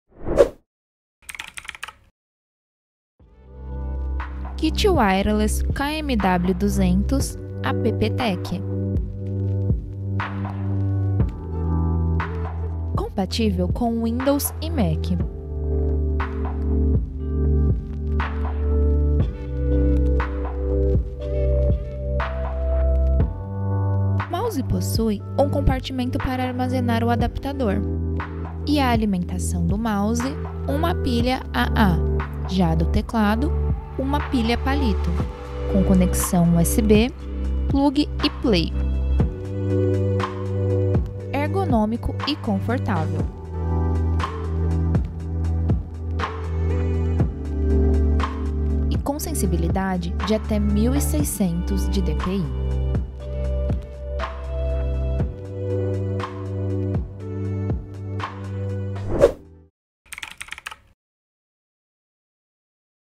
Potência e Conforto: Apresentando o Kit Wireless Preto KMW200 da APP-TECH
Prepare-se para uma revolução na sua experiência de computação com o Kit Wireless Preto KMW200 da APP-TECH! Este conjunto avançado de teclado e mouse sem fio combina potência, conforto e estilo para proporcionar um desempenho excepcional em todas as suas tarefas.

Em nosso mais recente vídeo, mergulhamos fundo nas características inovadoras do KMW200, desde a conectividade sem fio de alta velocidade até a ergonomia projetada para longas sessões de uso. Com controles intuitivos e uma resposta suave, este kit foi desenvolvido para atender às demandas dos usuários mais exigentes.

Assista agora para descobrir por que o Kit Wireless Preto KMW200 da APP-TECH é a escolha ideal para quem busca eficiência, conforto e estilo em sua estação de trabalho. Eleve sua produtividade e experimente o próximo nível de liberdade sem fio hoje mesmo!